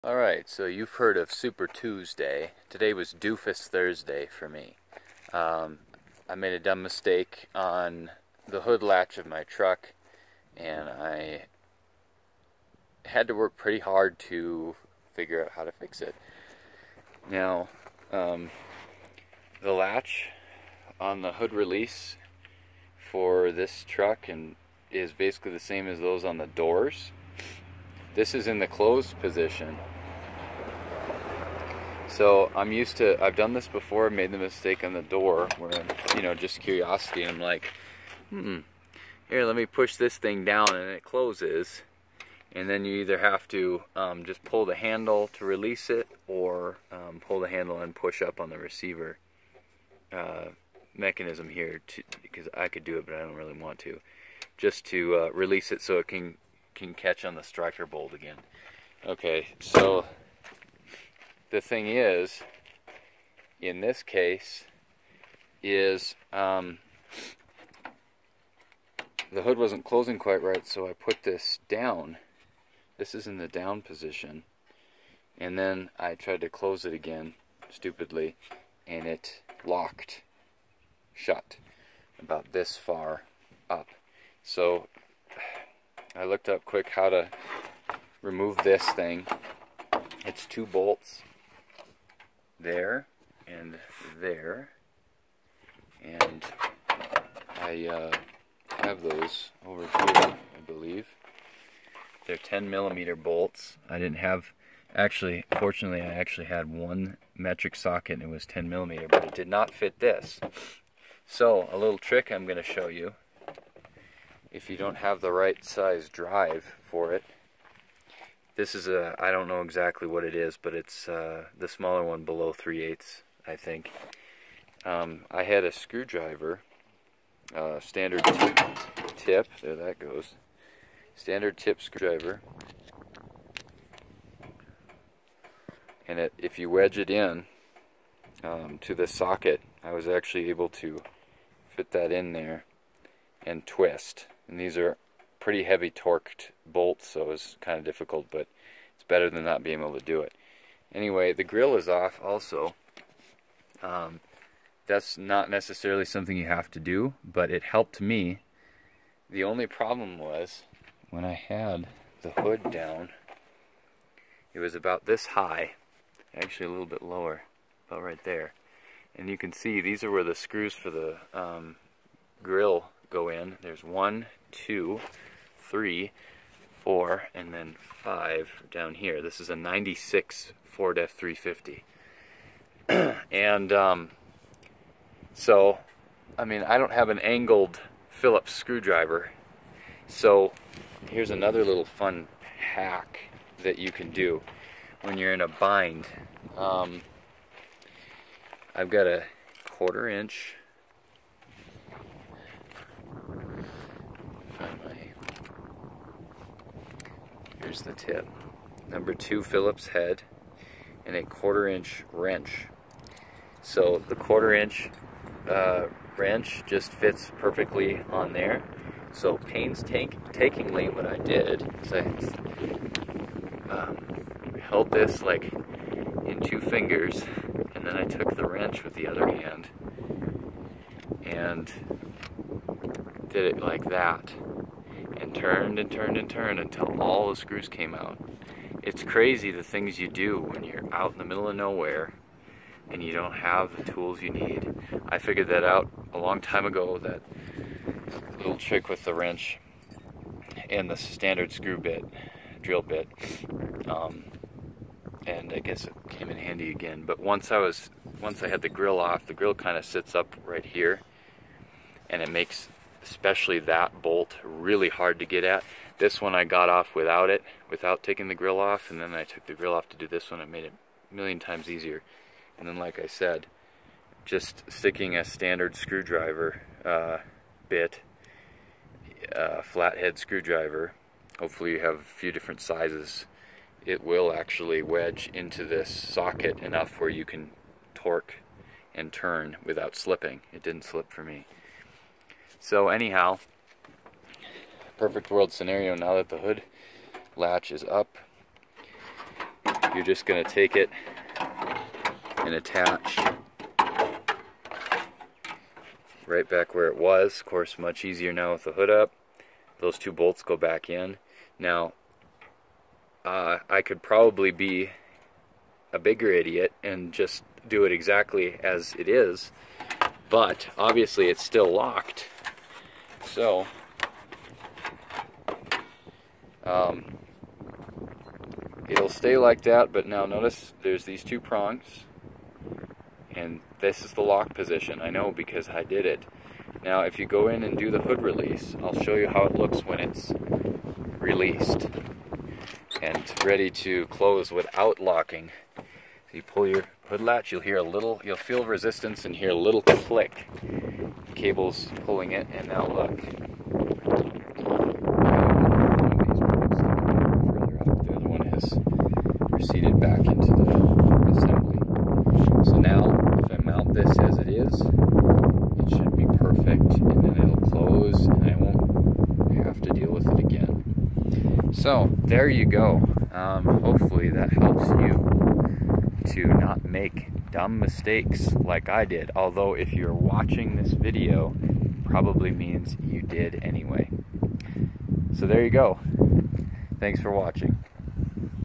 How to Release a Locked Stuck Hood Latch on a Ford Truck (Vlog)
Mine is a 1996 F-350 but this info should apply to all OBS (Old Body Style) F-series trucks for at least the 90s.

This problem occurs when you accidentally push down on the latch receiver and attempt to close the hood. The hood then gets stuck about 1" open and cannot be released using the hood release lever inside the vehicle. Removal of the receiver assembly is then required... Oh, joy!

***Make sure to note the two little hacks you can use when you have a limited set of tools! My struggle and investment of an inordinate amount of time can be your gain!***